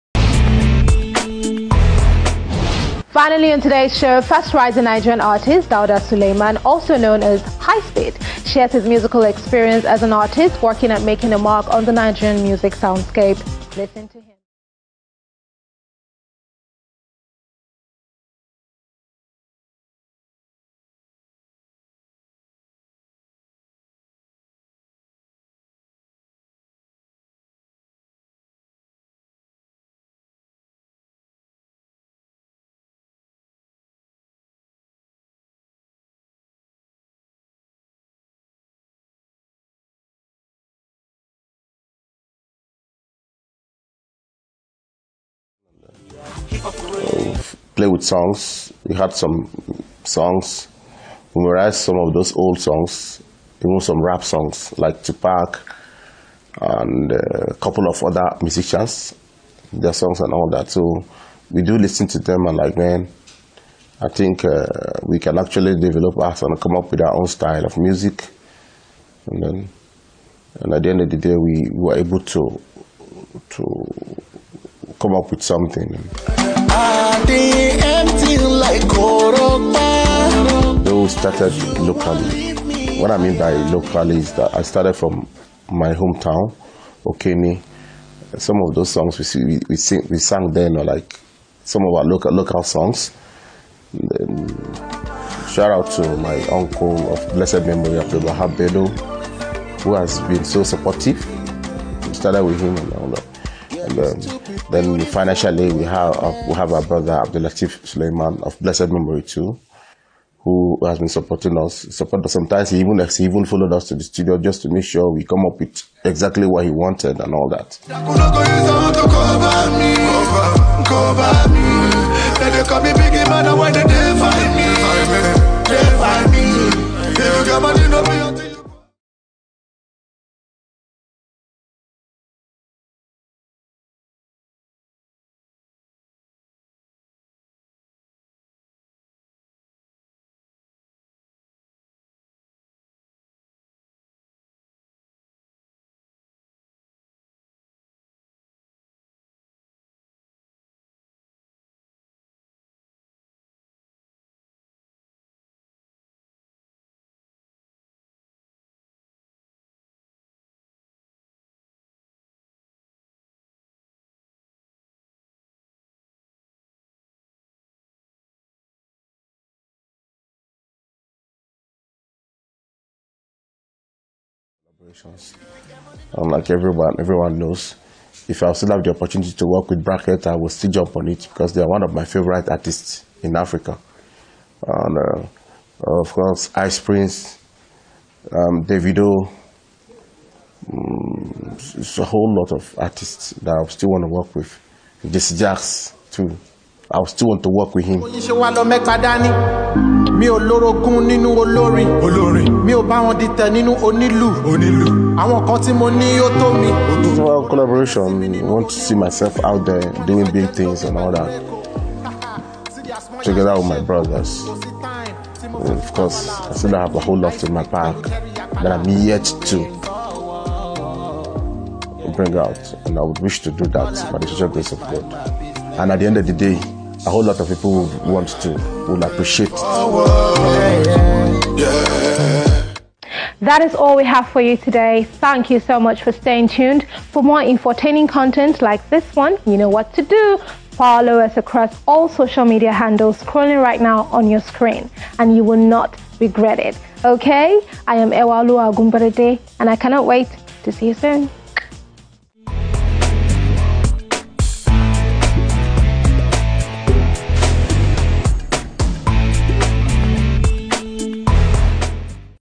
From Humble Beginnings to Stardom: My Interview Reveals the Untold Story of My Musical Journey
I recently had the honor of sitting down with Channels TV for an exclusive interview, where I opened up about the incredible journey of my musical career - from its humble beginnings to where we are today.

I want to honestly Appreciate everyone of you for the support thus far. God Bless You